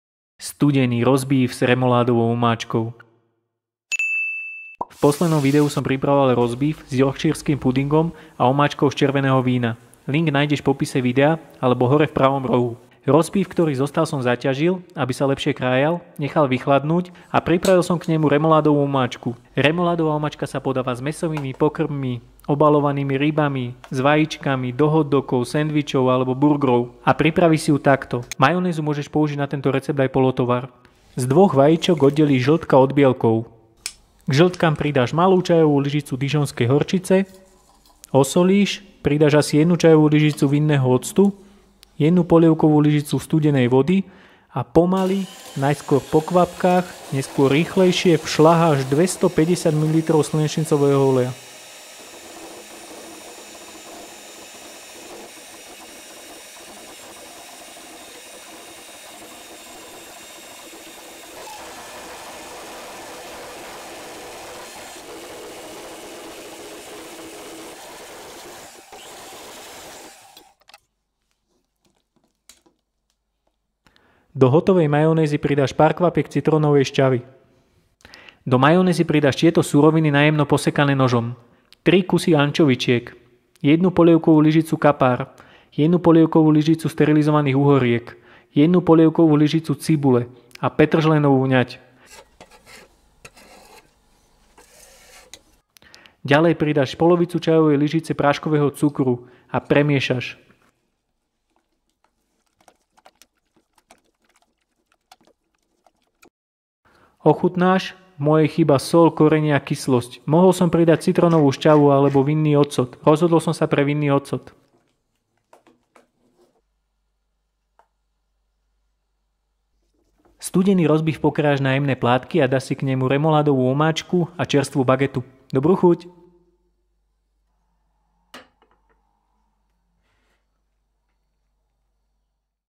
Studený rozbíf s remoládovou omáčkou.
V tomto videu si pripravíme rozbíf na studeno s remoládovou omáčkou.
Recept pre 6-8 porcií:
Majonéza:
-2 ks žĺtok
-1/2 ČL dijónska horčica
-1 PL voda
-250 ml slnečnicový olej
-citrónová šťava
-soľ
-asi 1 ČL vinný ocot

Remoláda:
-Majonéza
-1 PL sterilizované uhorky
-1 PL kapary
-3 ks ančovičky
-1 PL cibuľa
-1/2 ČL práškový cukor
-petržlenová vňať
-soľ,korenie
-vinný ocot alebo citrónová šťava